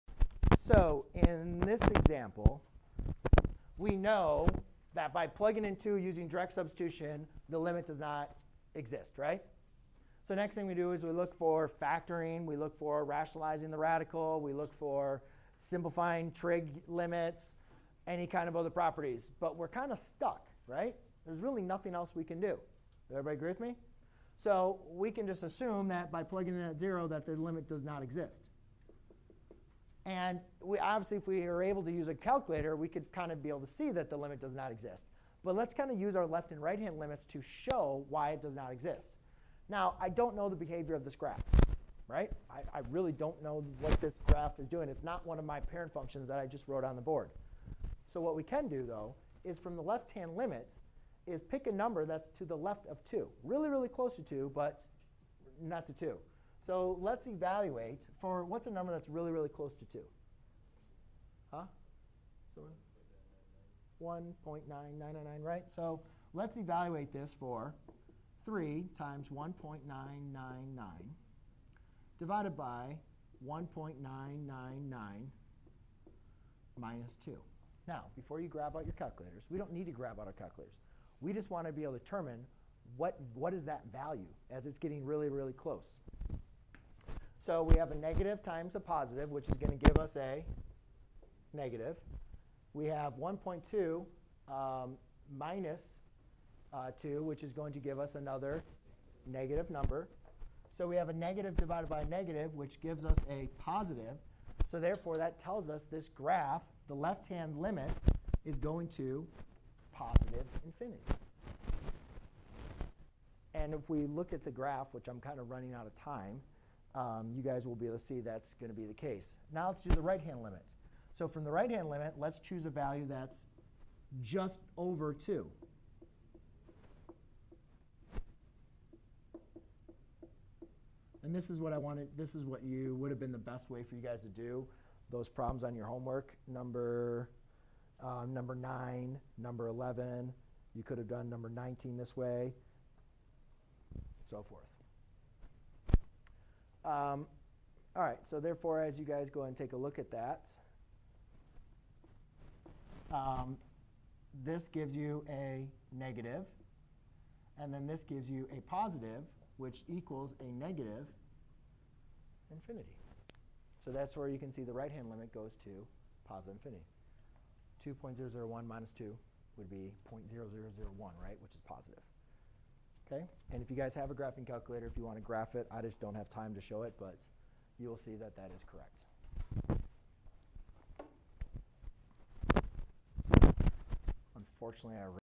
How to find the left and right hand limit by not using a calculator
👉 Learn how to evaluate the limit of a function involving rational expressions. The limit of a function as the input variable of the function tends to a number/value is the number/value which the function approaches at that time.
The limit of a function is usually evaluated by direct substitution of the value which the variable tends to. When the function is a rational expression such that direct substitution leads to zero in the denominator, we find a way to either eliminate the denominator by multiplying both the numerator and the denominator by a common factor or decompose the denominator and the numerator into constituent parts so that like terms can cancel out.